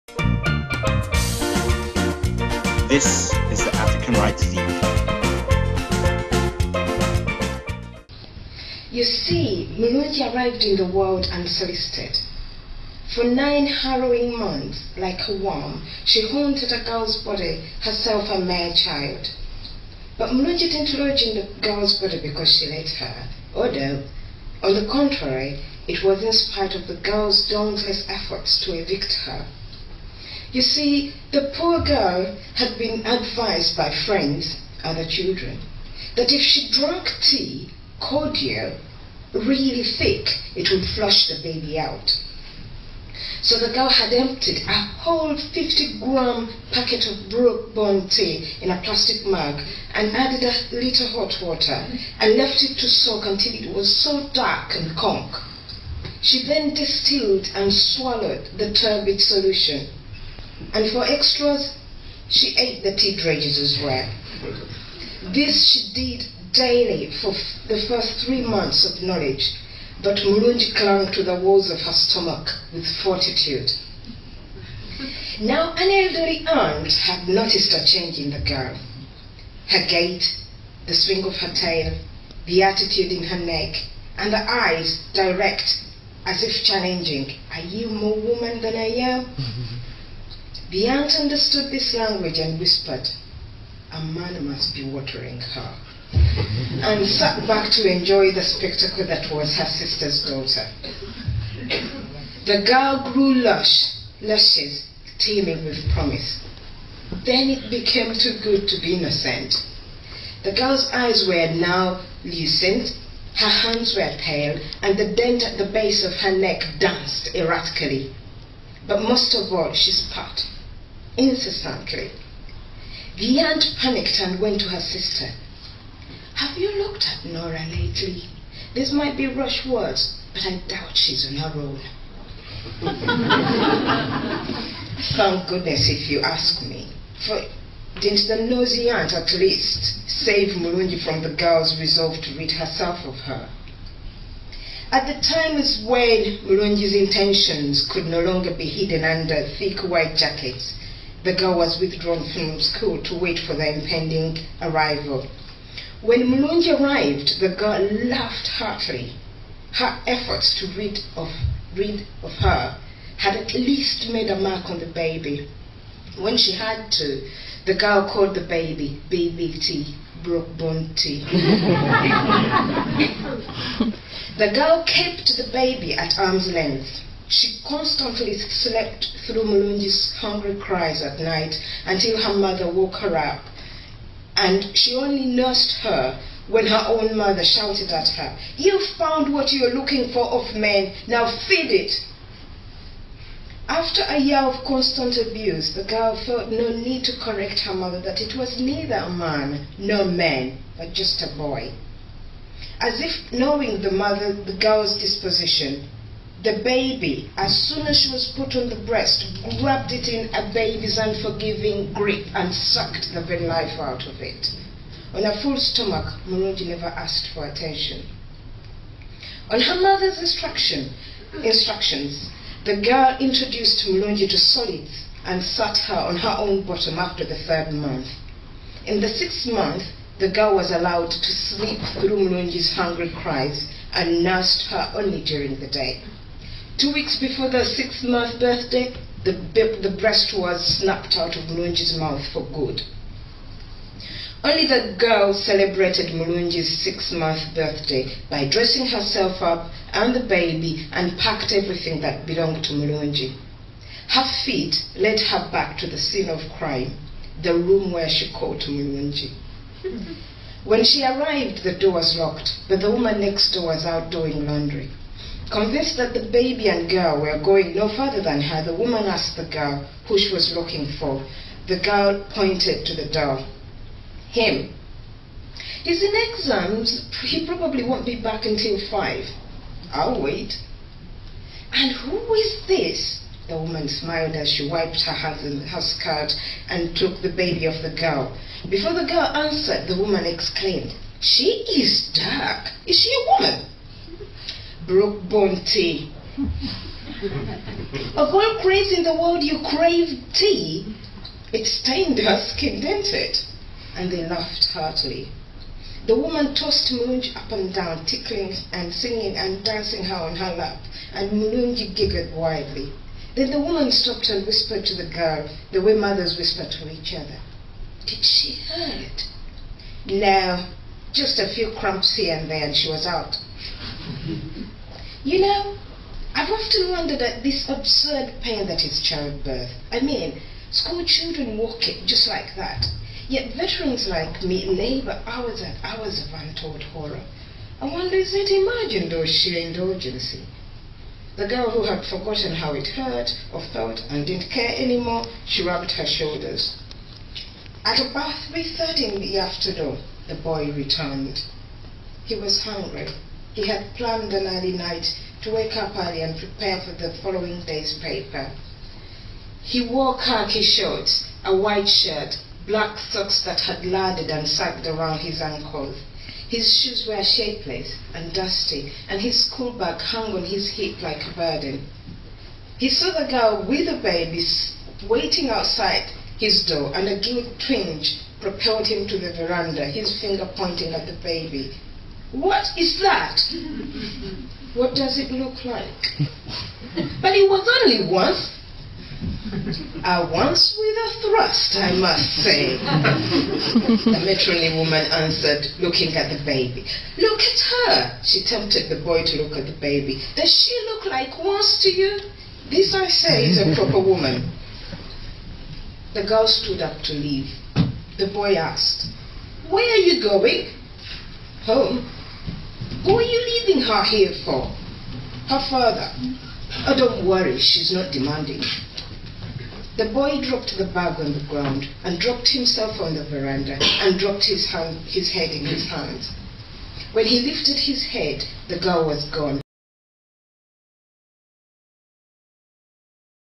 Jennifer Makumbi - June 2008 feature @ Southbank Centre
Jennifer reads from her unpublished novel, 'An Odd Equitorial Girl' during her featured set at the African Writers' Evening ] 5th anniversary reading at the Royal Festival Hall (London) in June 2008.